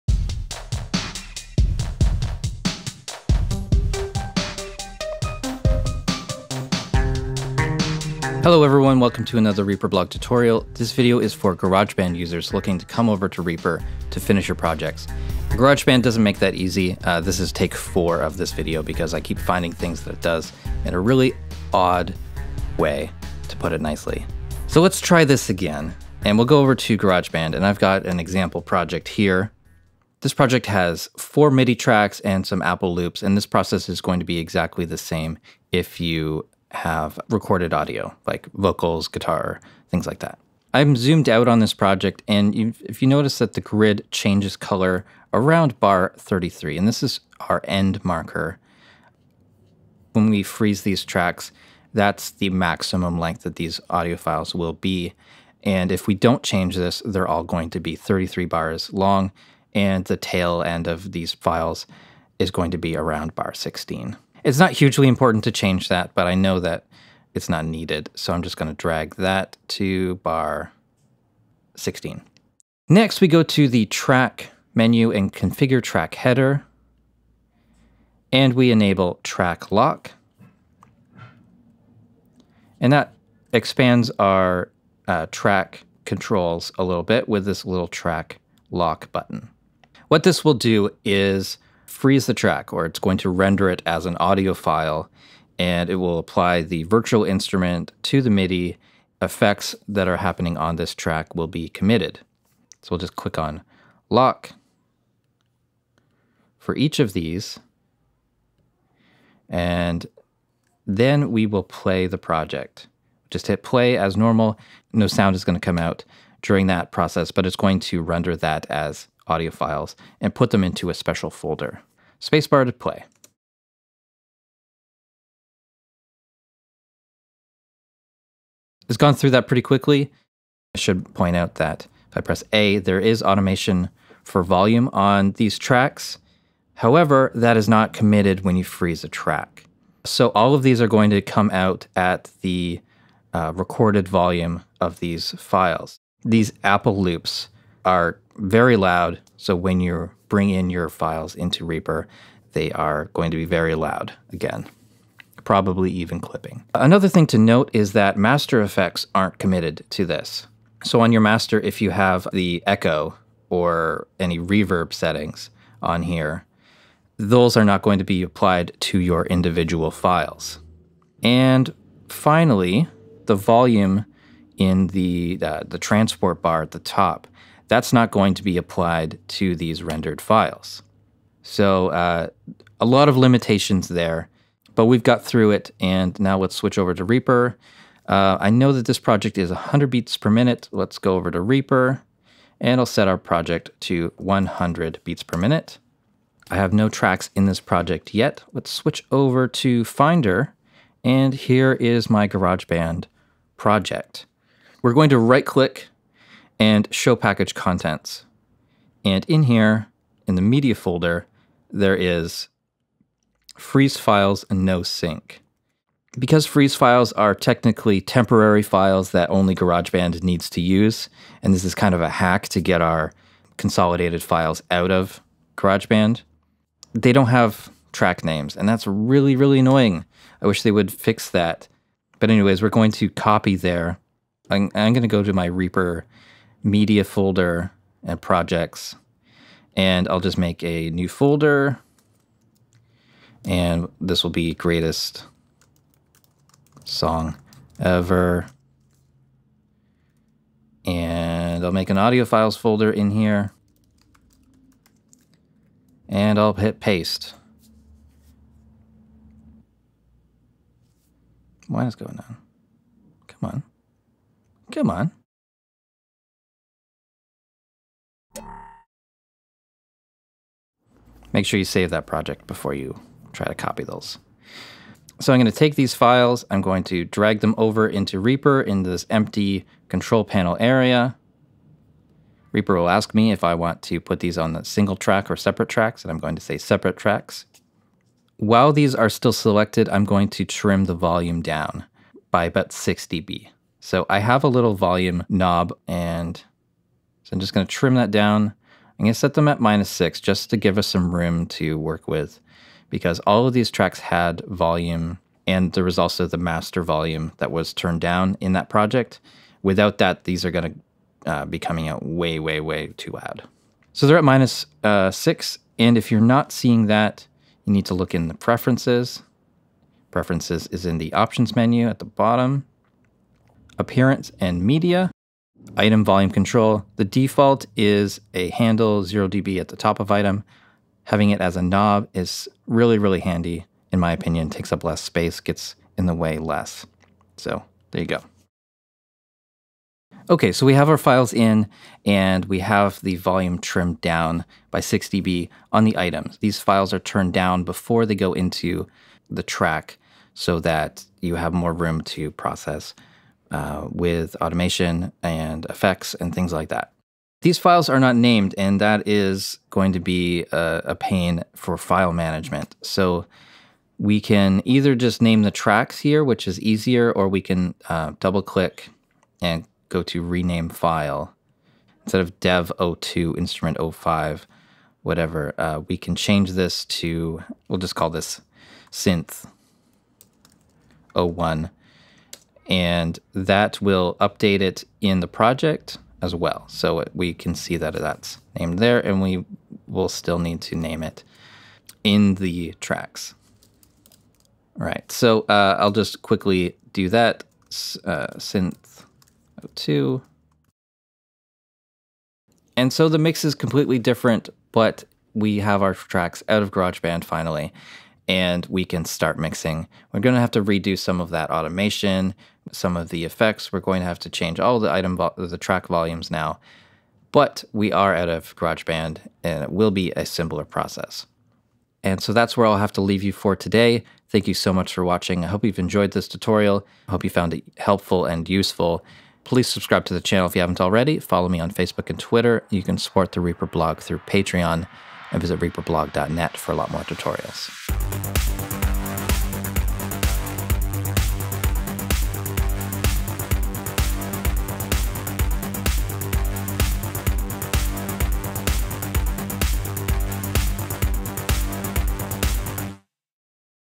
Transferring a song From Garageband To REAPER
A tutorial for Garageband users that want to move their song to REAPER.

Learn the trick to exporting stems from Garageband using the track lock/freeze function.

1 - set project end marker
2 - track menu: configure track header, enable lock button
3 - click lock button on each track
4 - PLAY - Garageband will begin freezing all tracks (rendering audio files)
5 - save project
6 - Find Garageband project in Finder (usually in username/Music)
7 - Right-click on project, choose "Show package contents"
8 - in media folder, find "freeze files no sync" - this is the rendered audio
9 - copy this audio to a folder for the REAPER project
10 - Name audio files
11 - import audio
12 - adjust item volume for headroom

Garageband really doesn't make it easy and you lose a lot in this process. Your mix will likely sound very different
- no automation
- track volume not applied to audio
- master volume not applied
- no master fx like reverb
- track names not used for rendered audio